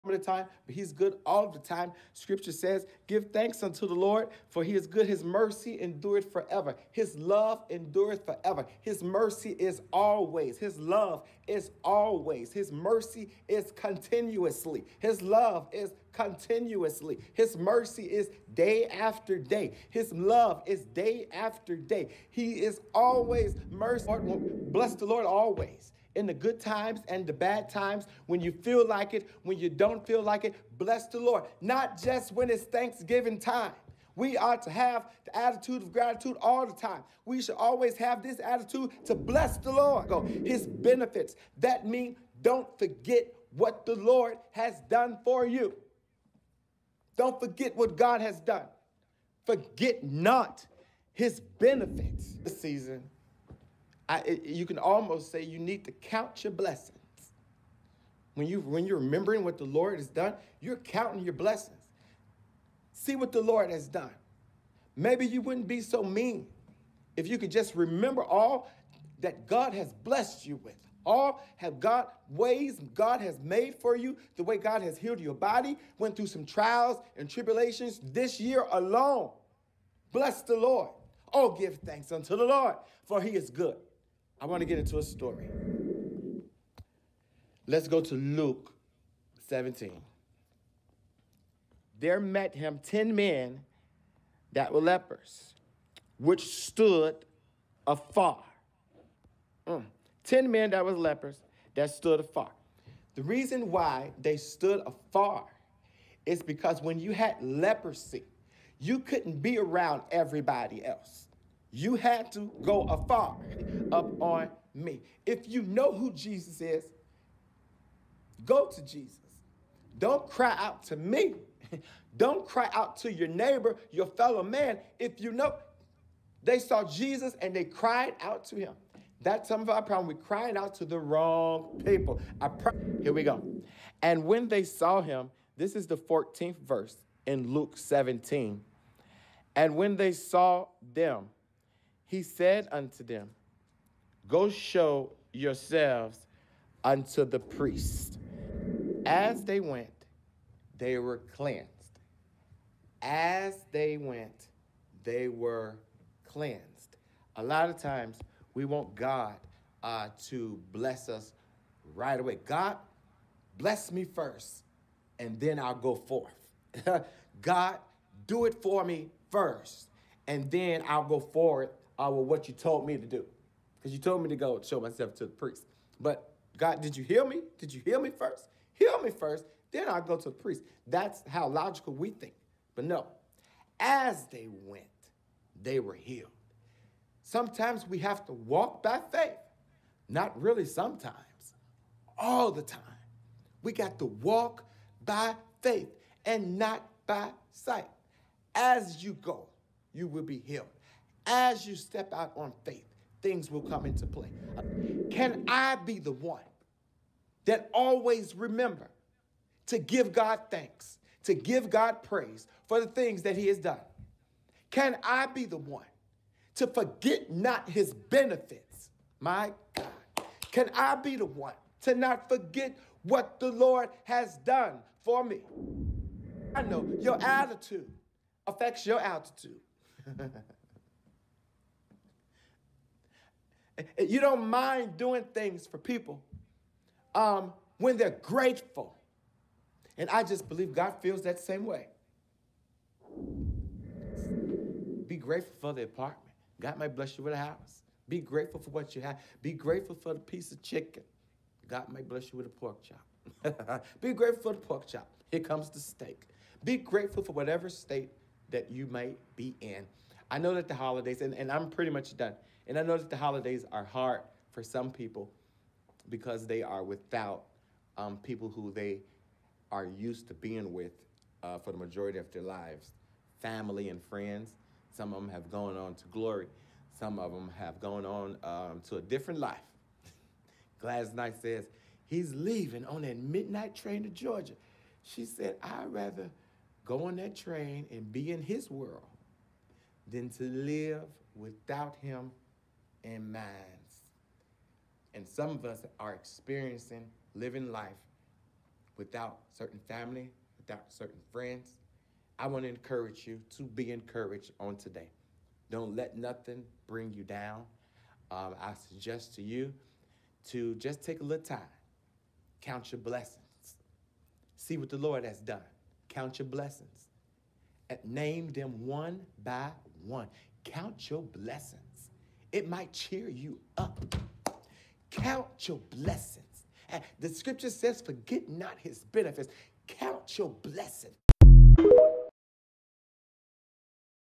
Bible Study Excerpts: Count Your Blessings - Pastor Daryl J. Fowler/ Seventh Star MBC
Virtual Bible Study Every Wednesday 7pm - Pastor Daryl J. Fowler - Seventh Star Missionary Baptist Church,

1322 West 87th street (Corner of 87th Ada) - Sunday Service 11am -

$7thStarMBC $DarylJF